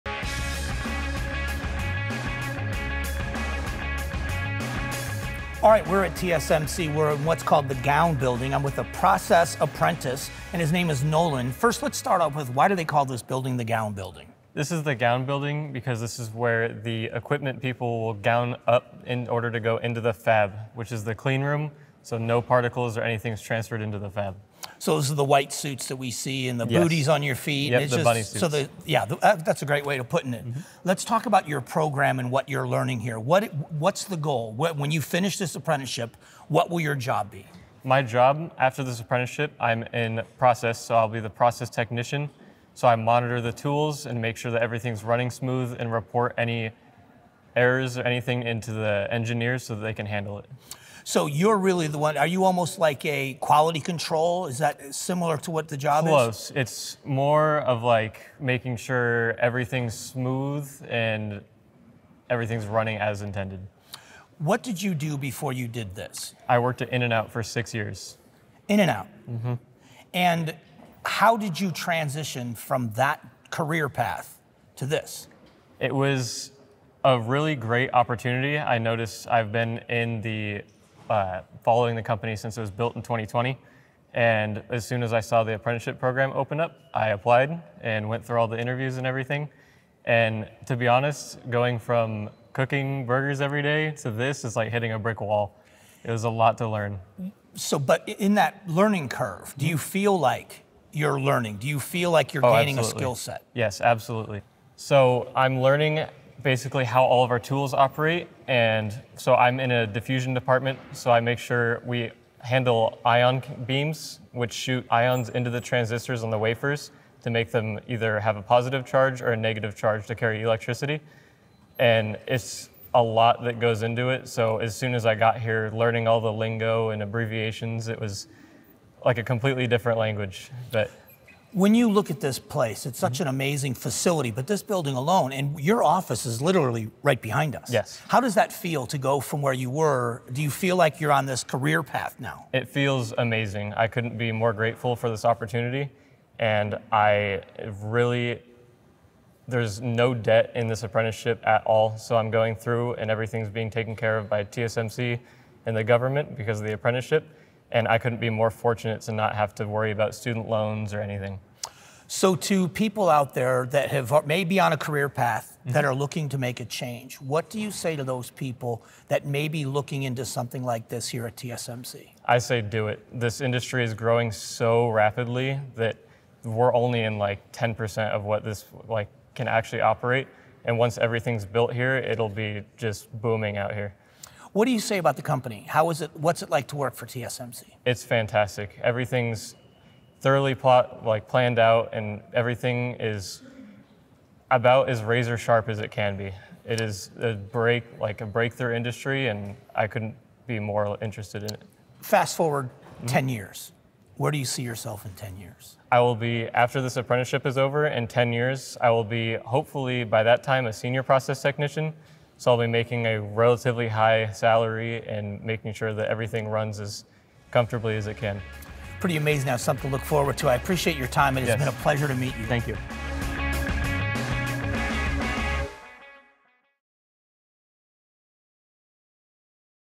Success Story: Young TSMC worker leave fast food job for career
Check out this success story. One of the youngest members of TSMC shares his transition from fast food to transistors.